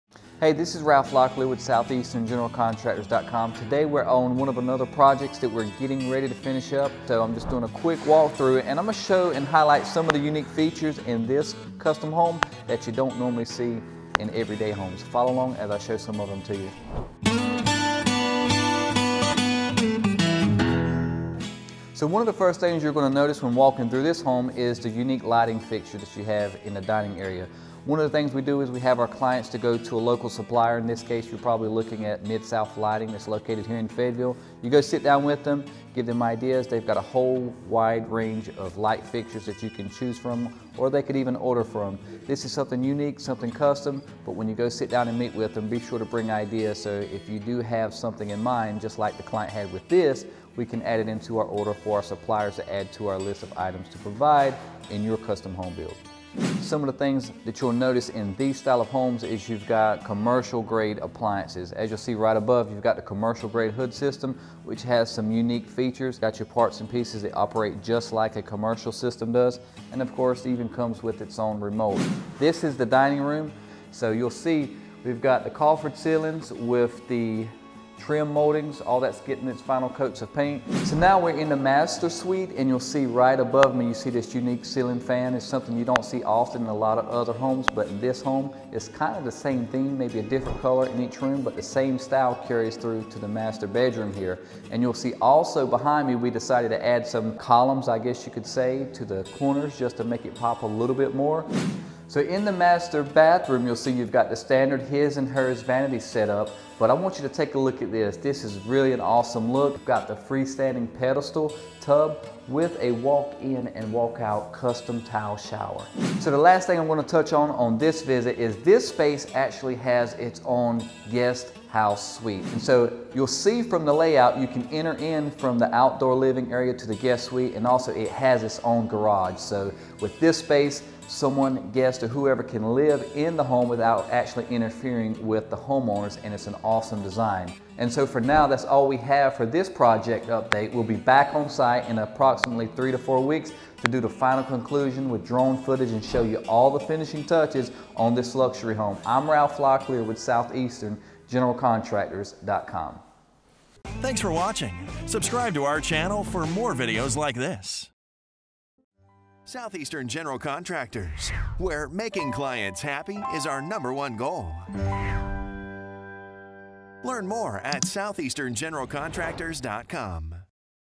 Custom Home Walkthrough | SEGC Vlog #52
Join Ralph, as he visits a luxury home in Fayetteville, NC that has been designed and constructed by South Eastern General Contractors.